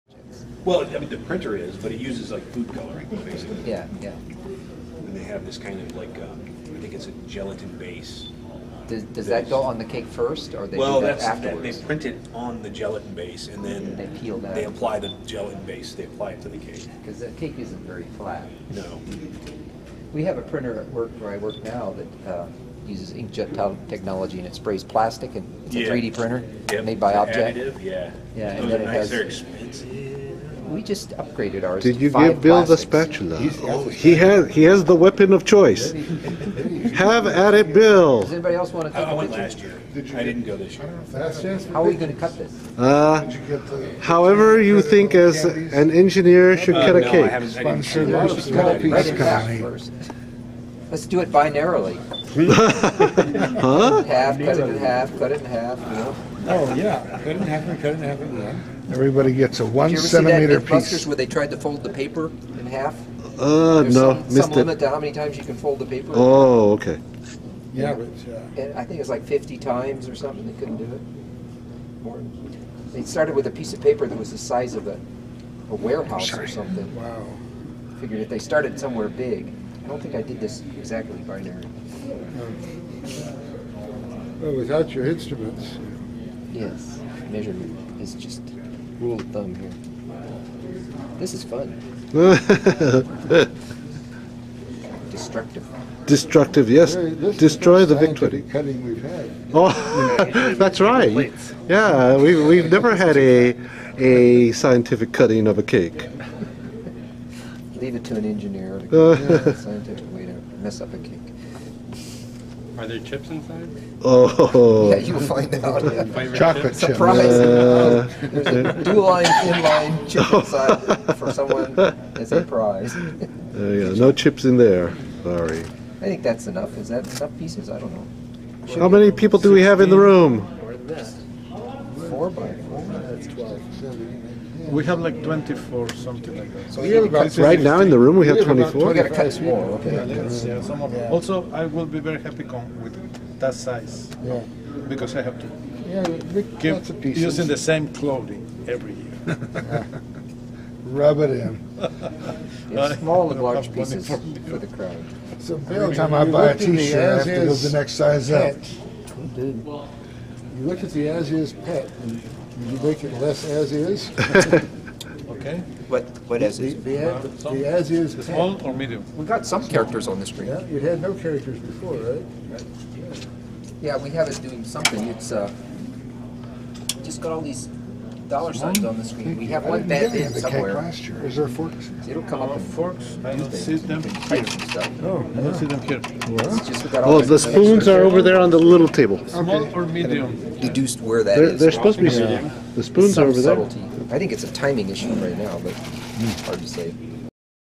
CommVEx cake and Bill Seiler reports on his PET 8032 repair
On Saturday, July 30, 2016, at the Commodore Vegas Expo v12, CBM engineer Bill Seiler sliced the 35th anniversary cake for the Commodore VIC-20.  He also reported on his progress on his repair of a Commodore PET 8032 at the show.  Filmed by Robert Bernardo